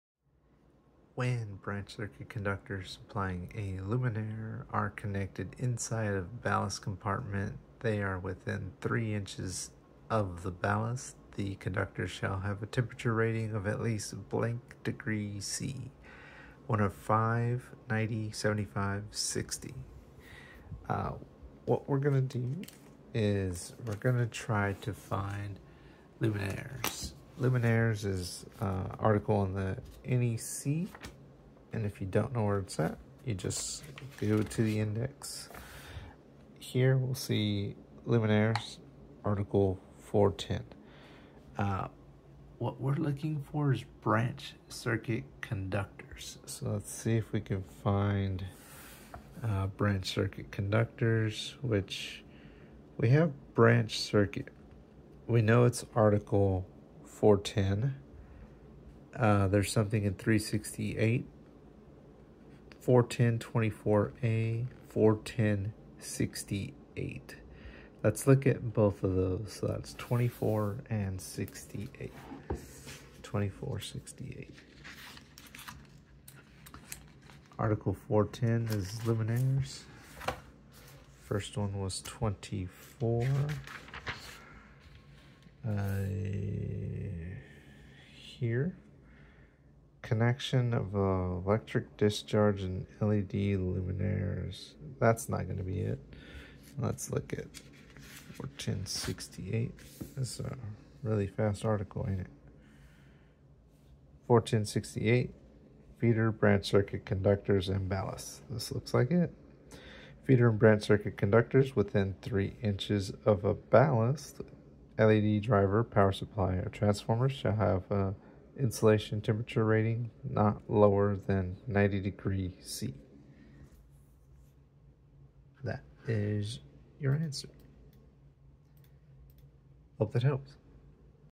NEC Luminaire Conductors - Exam Question ANSWERED!
When conductors are located near ballasts, the heat can rise fast — and if you don’t know the minimum conductor temperature rating required, it can lead to insulation damage or premature failure.

In this NEC exam-style question, we break down what the Code says about branch-circuit conductors inside luminaire ballast compartments and how to choose the proper conductor insulation for these high-temperature environments.

📋 Question Covered:
When branch-circuit conductors supplying a luminaire are contained inside a ballast compartment and they are within 3-inches of the ballast, the conductors shall have a temperature rating of at least _______ °C.
A. 105
B. 90
C. 75
D. 60

📖 Where to Look in the 2023 NEC:

Keywords to Search: Luminaires, Branch-Circuit Conductors

Relevant Code Section: 410.68 – Feeder and Branch-Circuit Conductors and Ballasts

💡 Pro Tip:
Always verify conductor insulation ratings when wiring fixtures — heat buildup around ballasts and drivers is one of the most common causes of insulation breakdown in lighting systems.